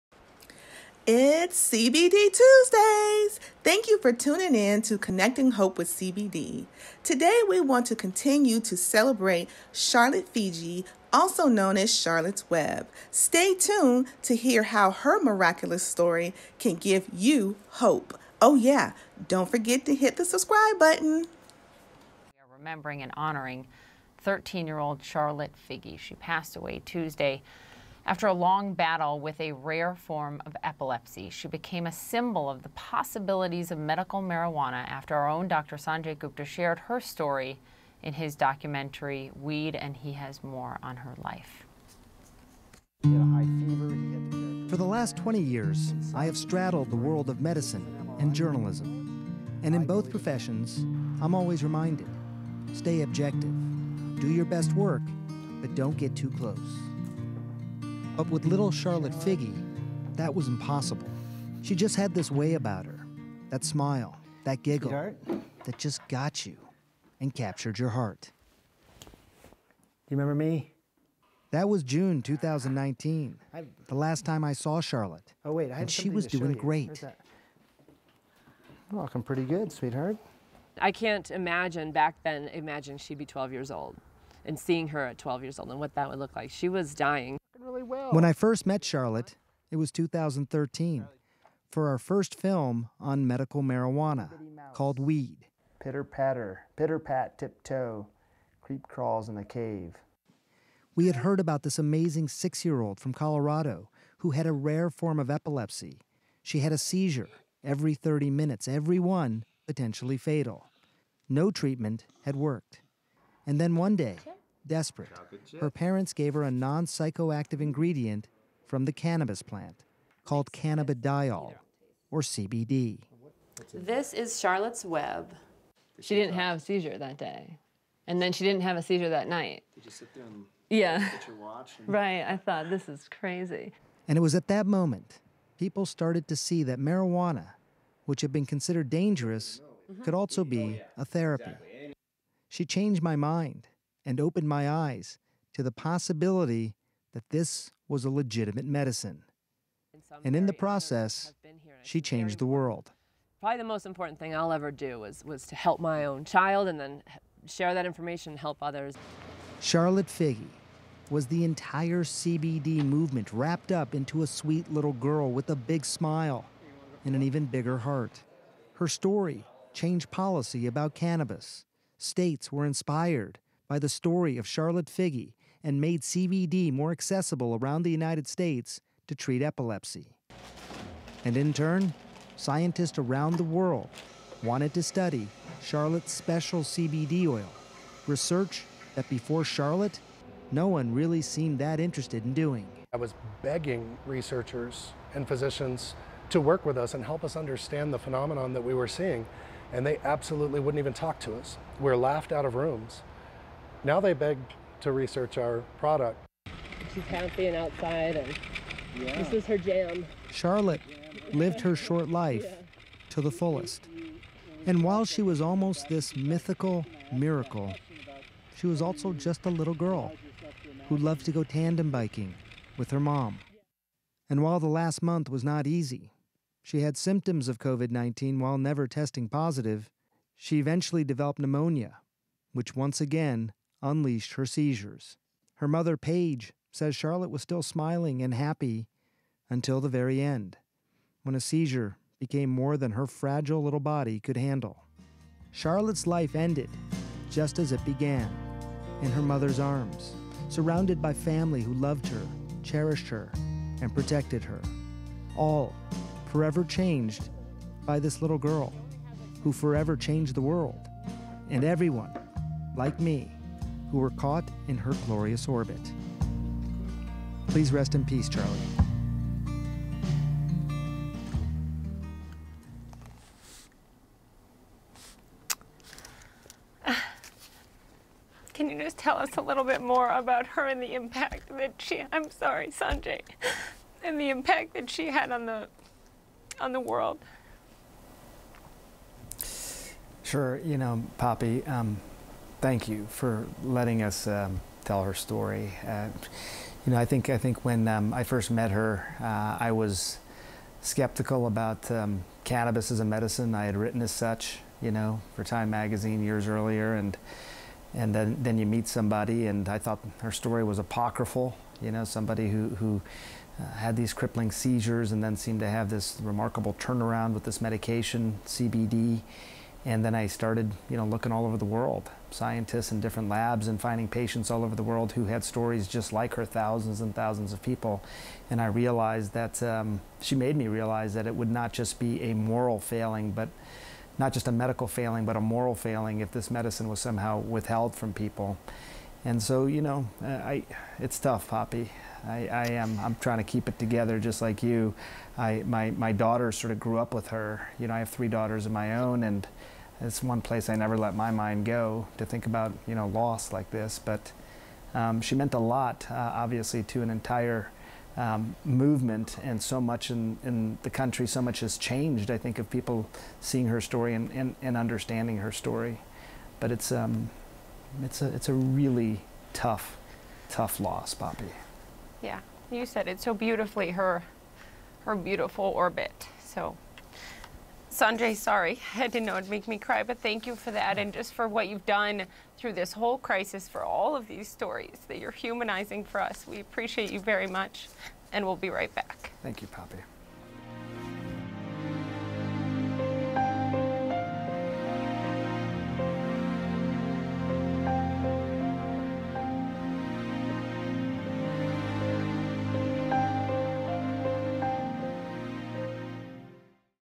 Connecting Hope With CBD (Remembering Charlotte's Web)
CBDTuesdays ConnectingHopeWithCBD TheHopeConnections ...This episode of CBD Tuesday's is in memory of Charlotte Fiji.